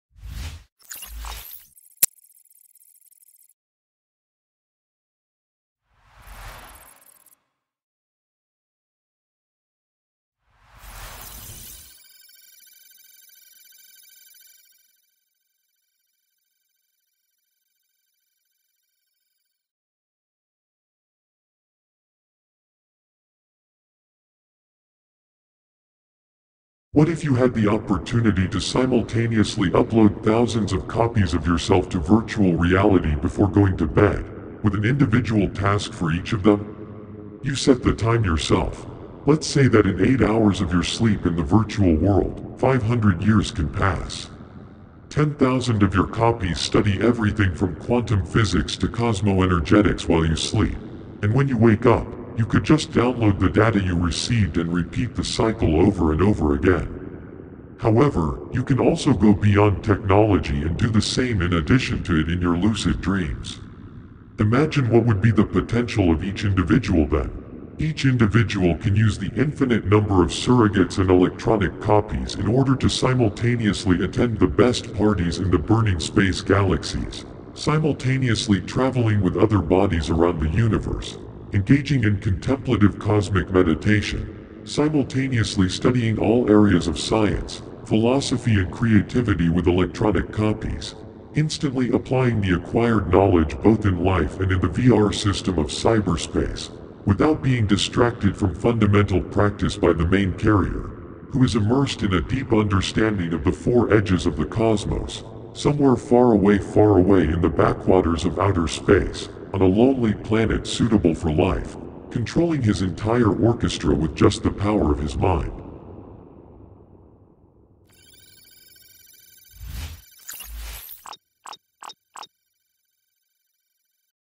Astral SpaceX: Cosmocybernetics. Multidimensional Intergalactic Communication.
What if you had the opportunity to simultaneously upload thousands of copies of yourself to virtual reality before going to bed, with an individual task for each of them? You set the time yourself. Let's say that in 8 hours of your sleep in the virtual world, 500 years can pass. 10 thousand of your copies study everything from quantum physics to cosmoenergetics while you sleep, and when you wake up, you could just download the data you received and repeat the cycle over and over again.